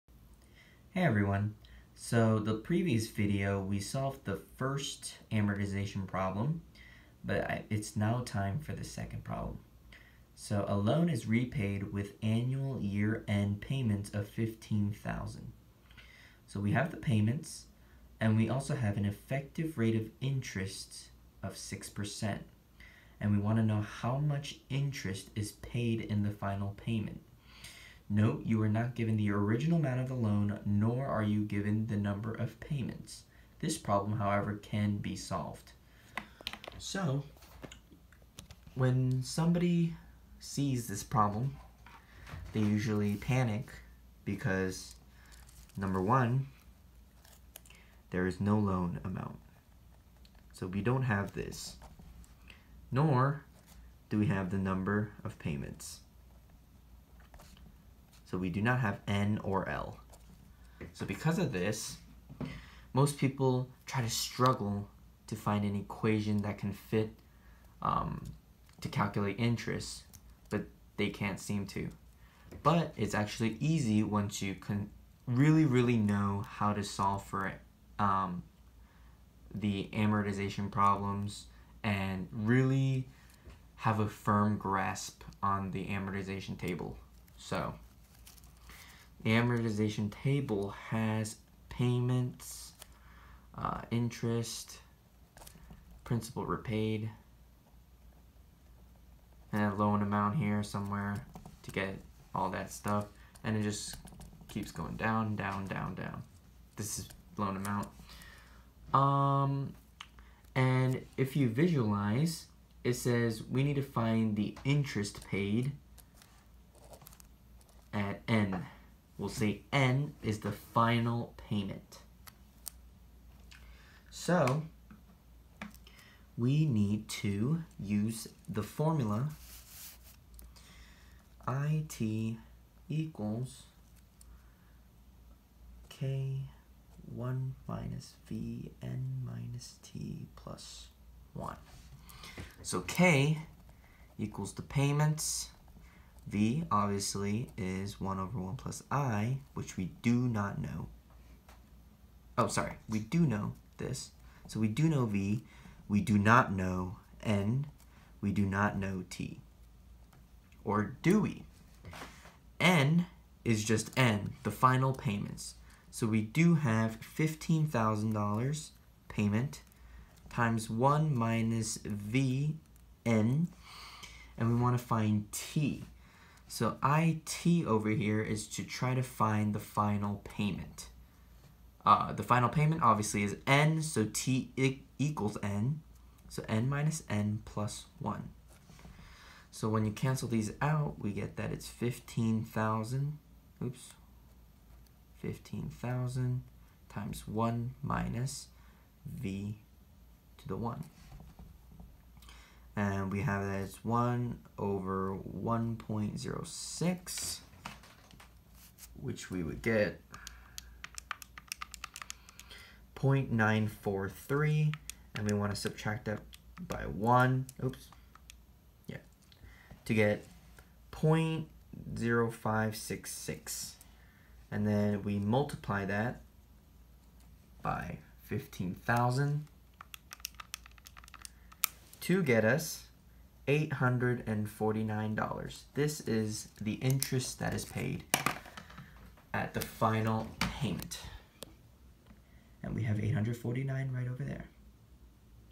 Solving FM exam problem #18 (using the Amortization table)
A challenging problem if you do not have a firm grasp of the Amortization table.